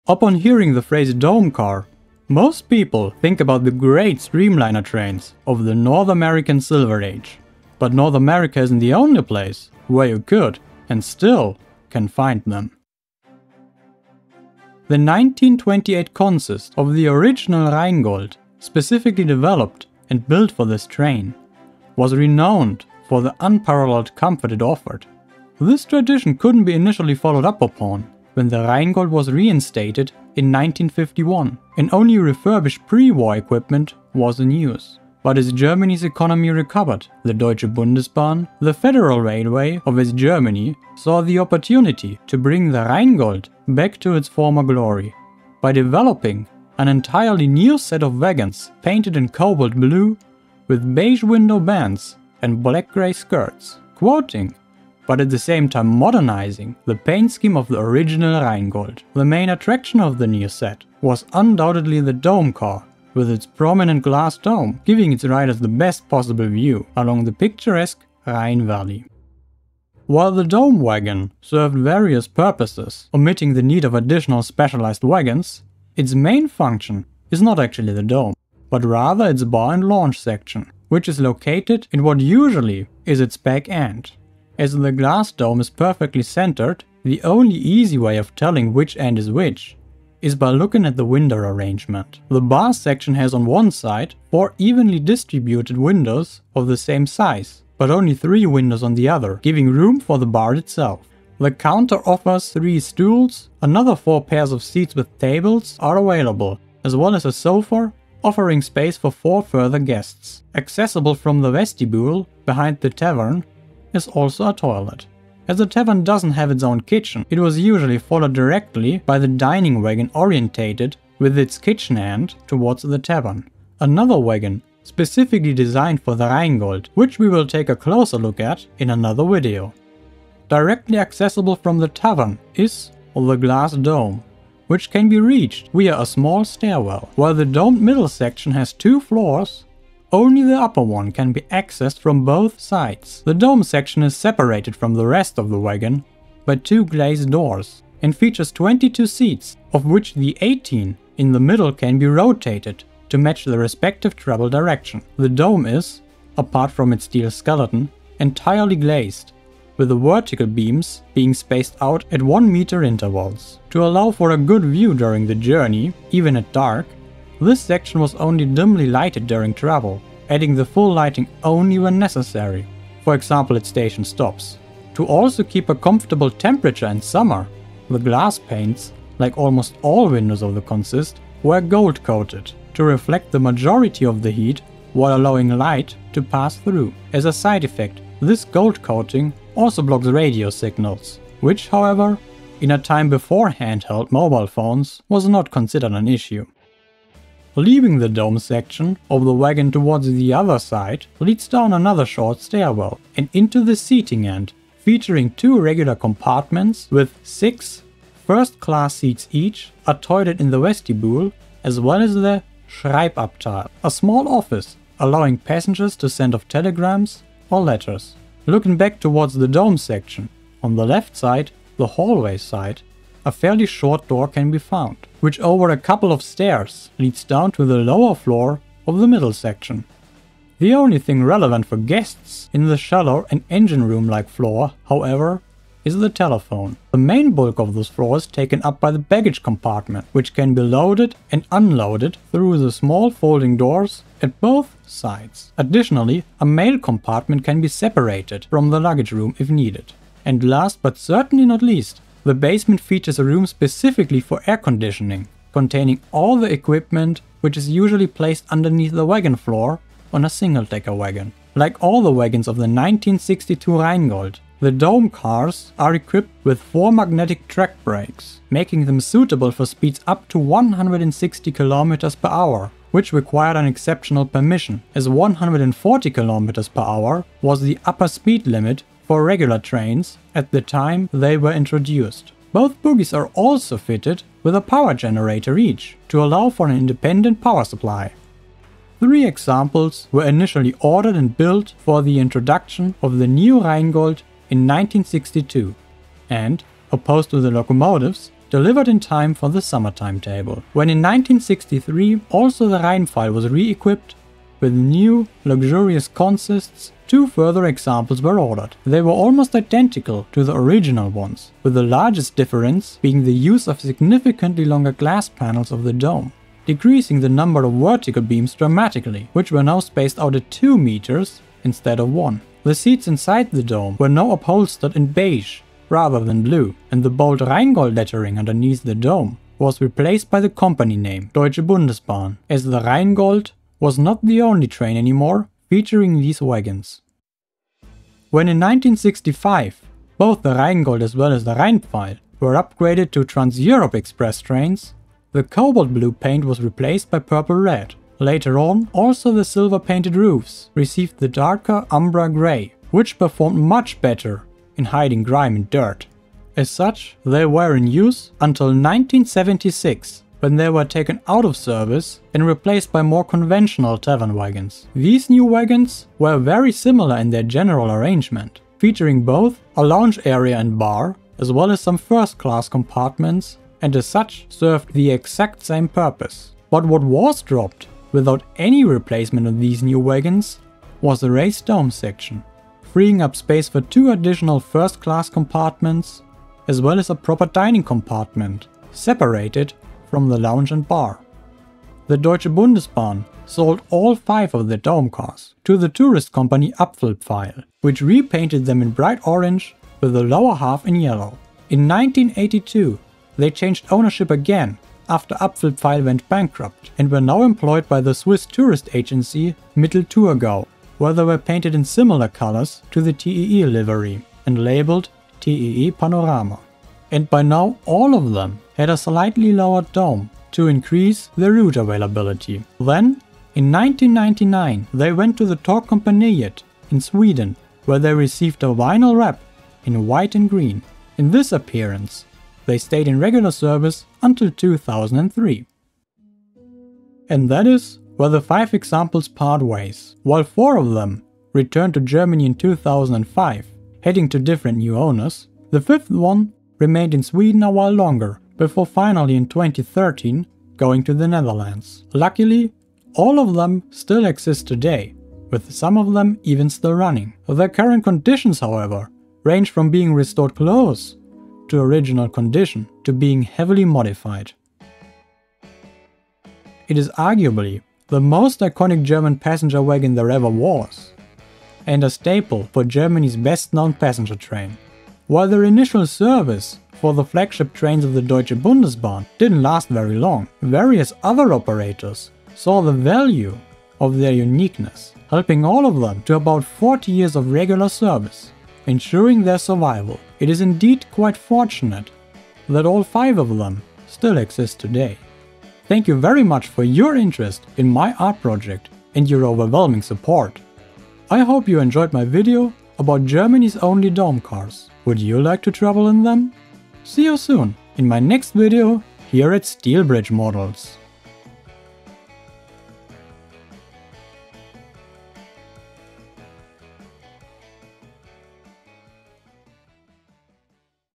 The only Dome Cars in Germany!
The most iconic piece of rolling stock in the 1962 Rheingold consist is without a question the unique dome car. But, how do they look from inside, and did any of them survive? Enjoy!

All models in this video were created and rendered by myself using Blender.

Chapters:

0:00 Intro
0:14 Act I: Origin
1:09 Act II: Interior
4:21 Act III: Service
5:56 Act IV: Afterlife
7:16 Act V: Heritage
7:46 Outro